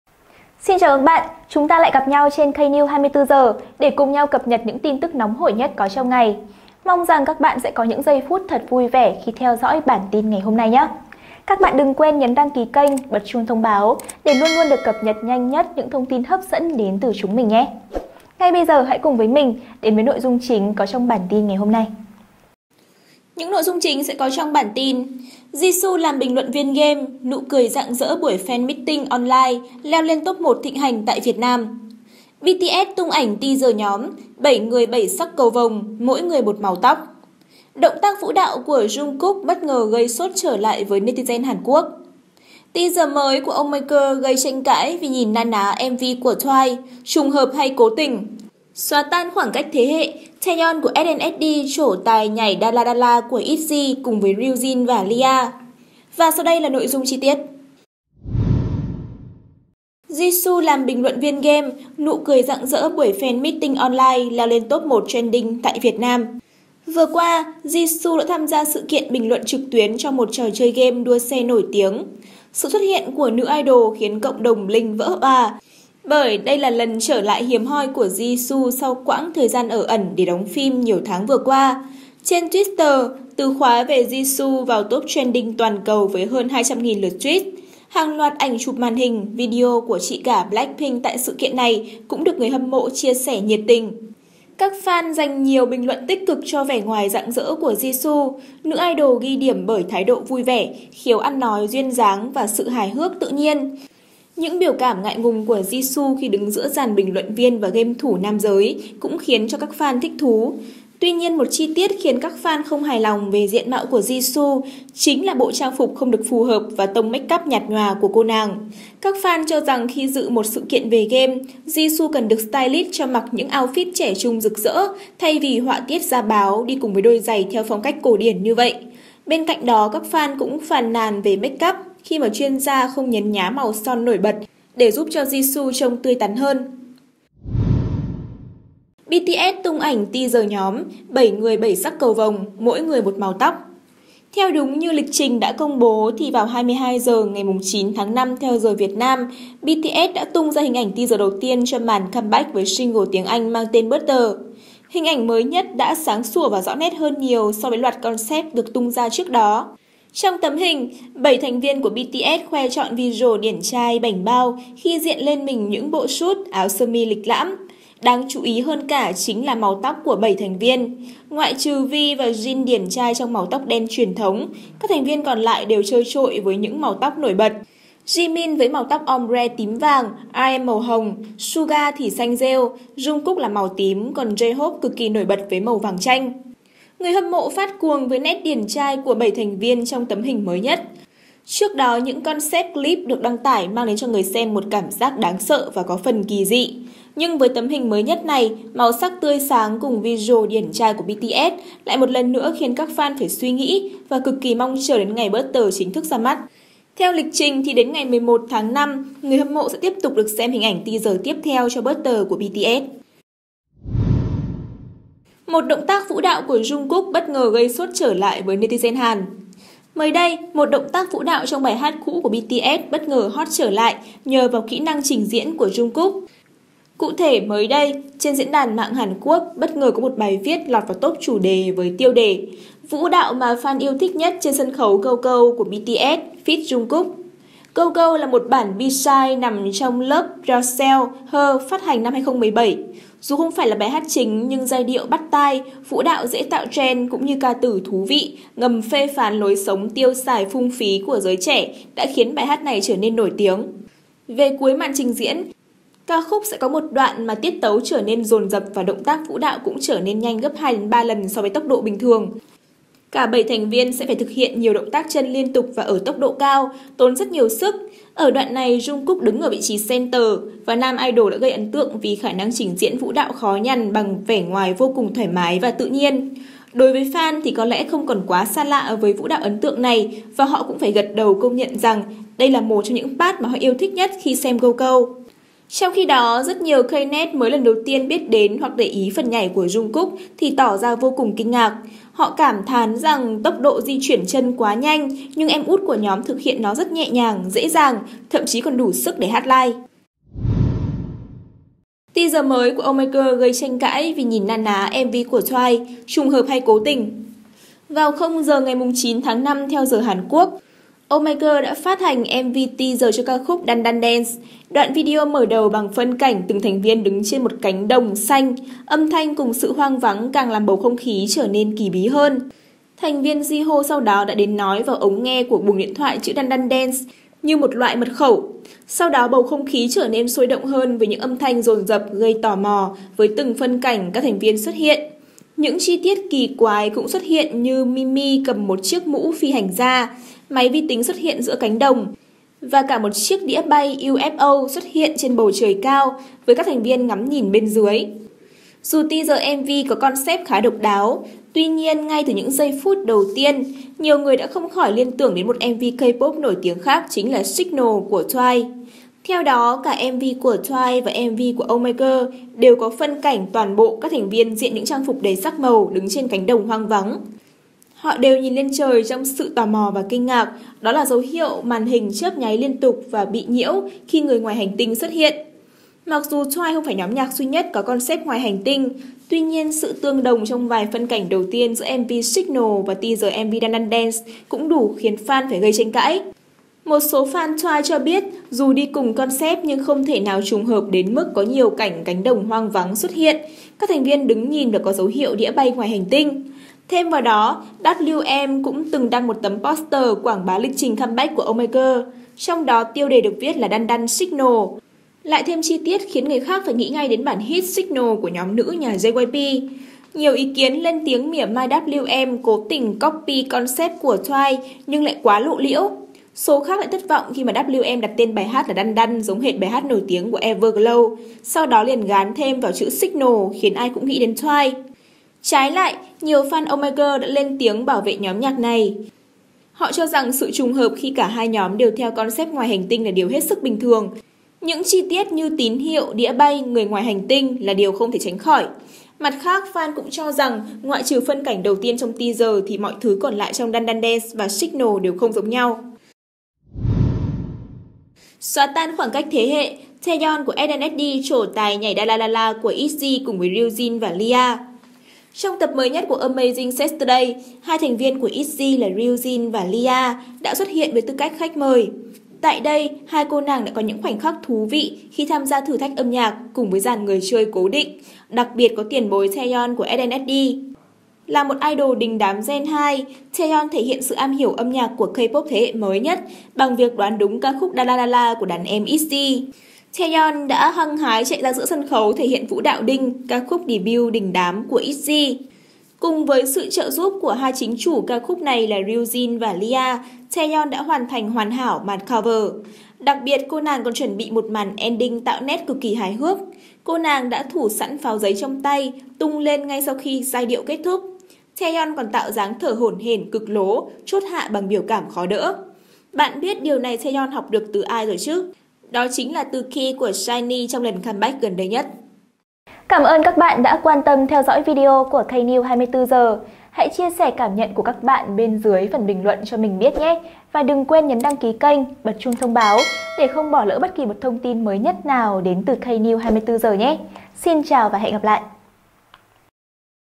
Jisoo Làm BLV Game Leo Lên Top 1 Trending Việt Nam; BTS Tung Ảnh Teaser Nhóm Mỗi Người 1 Màu Tóc
Jisoo Làm BLV Game Leo Lên Top 1 Trending Việt Nam; BTS Tung Ảnh Teaser Nhóm Mỗi Người 1 Màu Tóc | KNEWS 24H